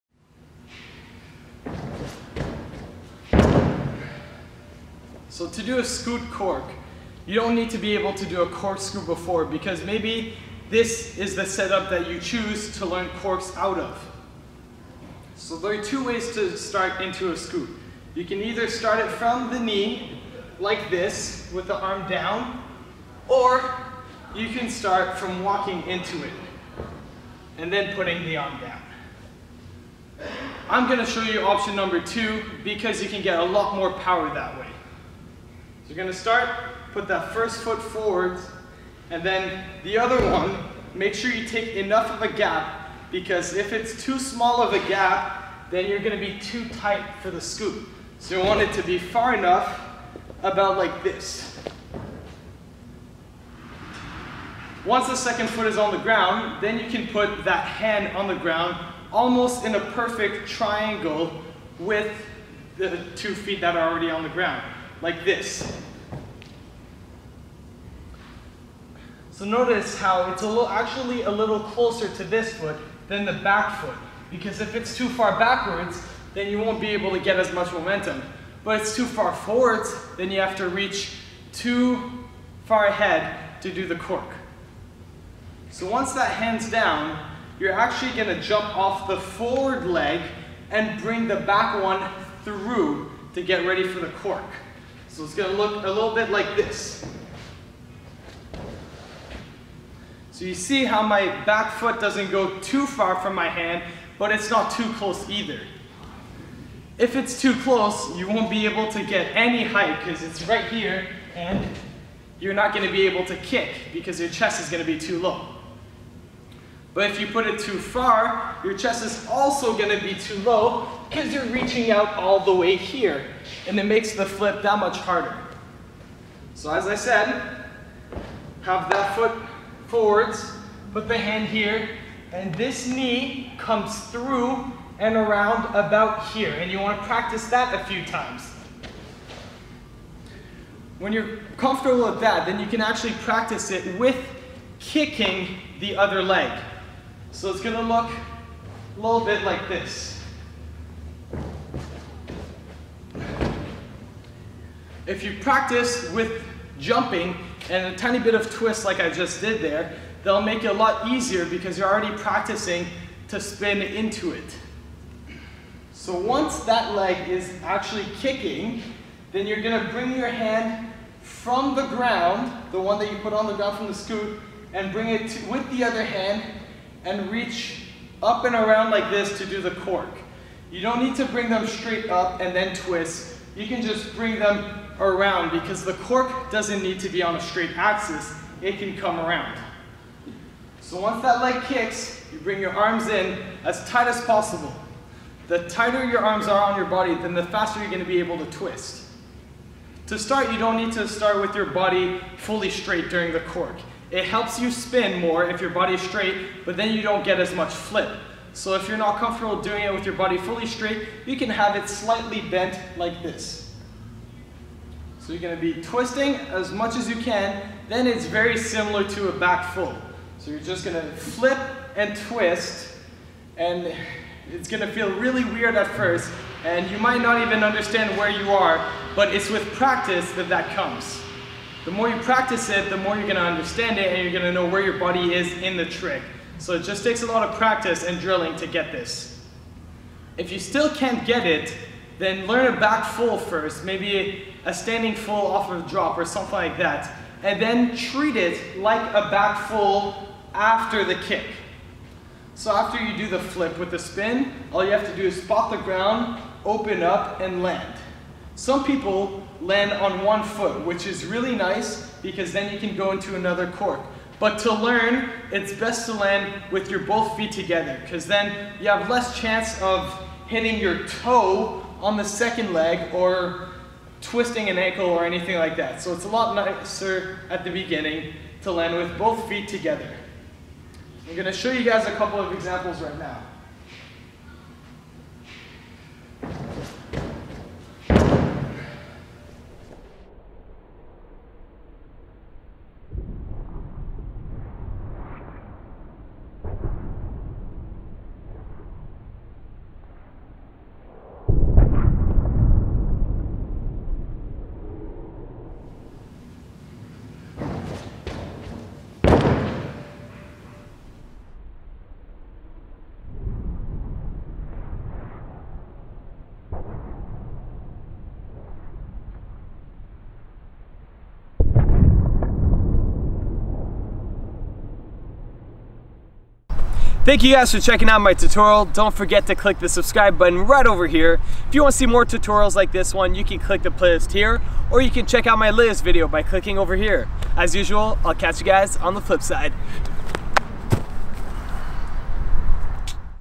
How to do a Scoot Corkscrew (Tricking, Parkour Tutorial)
How to do a Scoot Corkscrew (Tricking, Parkour Tutorial) | Nick Pro